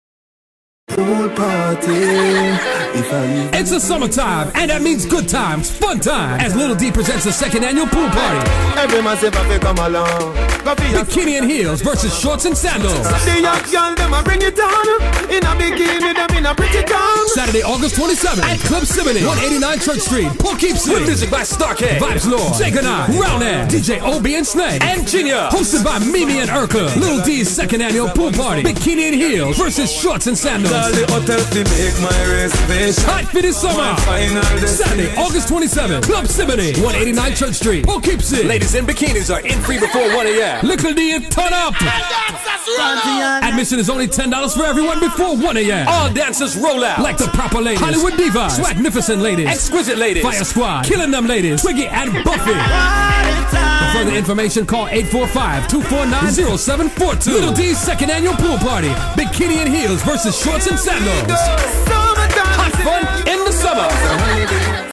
Lil D Summer Time Pool Party Aug 27th Ciboneys
DJ LIL D POOL PARTY AUG 27TH @ CIBONEY!!! LADIES GET THAT BODY RITE FOR THIS EVENT! BIKINI N HEELS VS SHORTS N SANDALS...ANY GIRL THAT HAVE TO TUCK IN THEM BELLY PLZ I REPEAT PLZZZZZZZ DO NOT WEAR A 2PIECE!